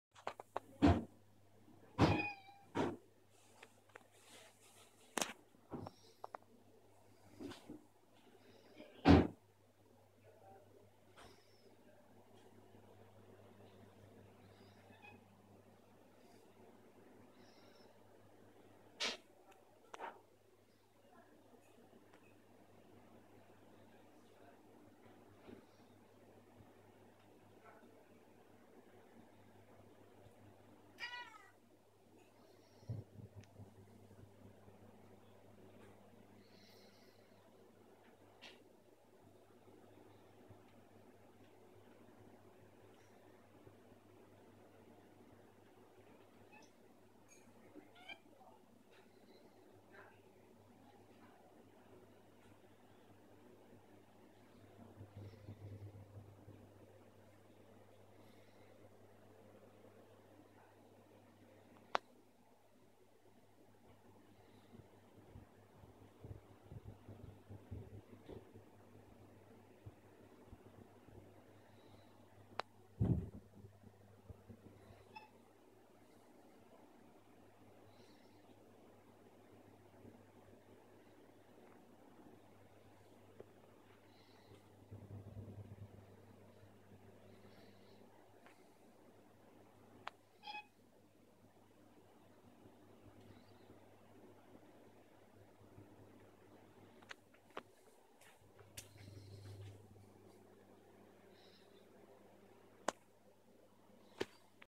Cat mews at ceiling bin bags
We stuck bin bags to the ceiling and the cats were confused.

UPDATE:
She's still meowing